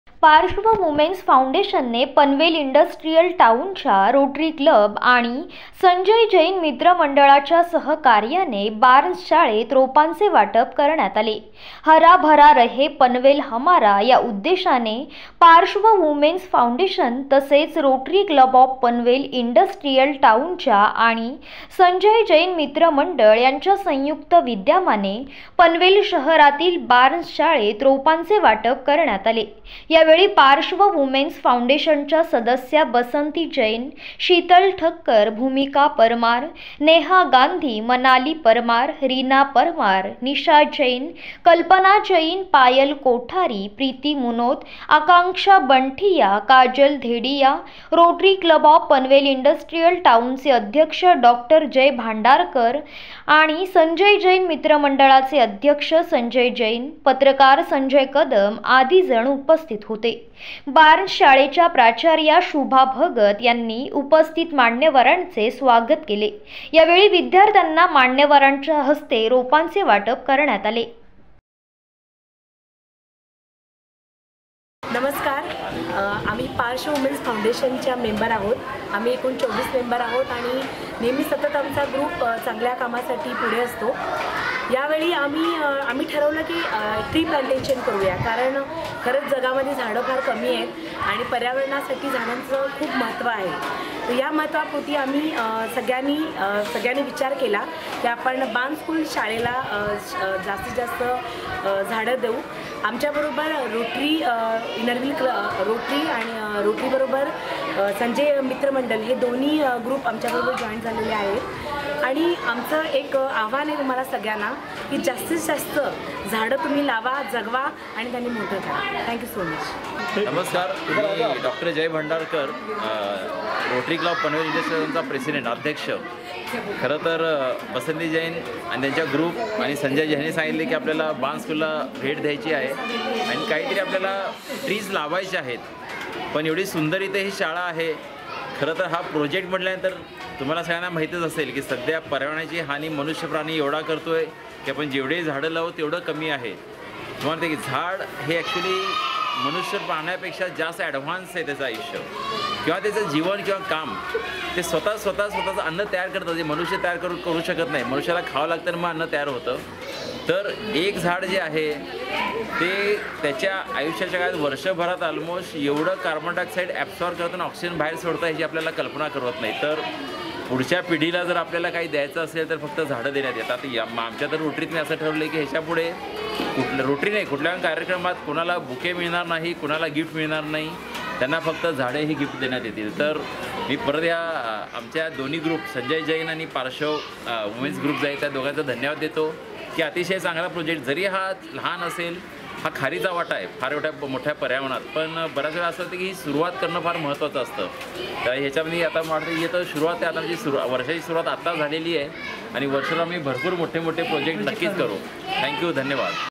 संजय जैन मित्र मंडळातर्फे बार्न्स शाळेत रोपांचे वाटप !!
पार्श्व वुमेन्स फाऊंडेशन, रोटरी क्लब ऑफ पनवेल इंडस्ट्रियल टाऊनच्या आणि संजय जैन मित्र मंडळातर्फे बार्न्स शाळेत रोपांचे वाटप#malharnews